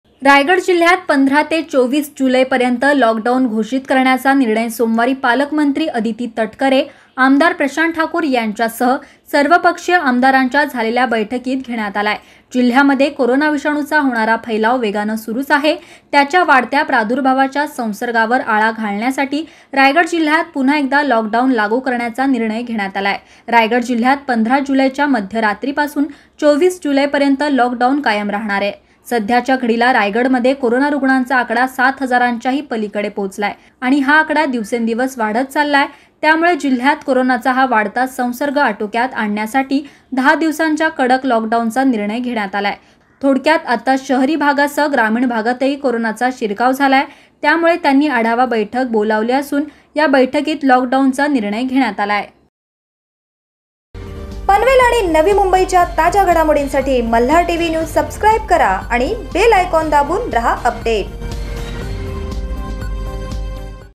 रायगड जिल्ह्यात पुन्हा लॉकडाऊन सर्वपक्षीय आमदारांच्या बैठकीत निर्णय !
malhartvnews at panvel | १५ जुलै मध्यरात्री पासून २४ जुलै पर्यंत लॉकडाऊन ;
रायगड जिल्ह्यात पुन्हा लॉकडाऊन सर्वपक्षीय आमदारांच्या बैठकीत निर्णय;पालकमंत्र्यांची घोषणा !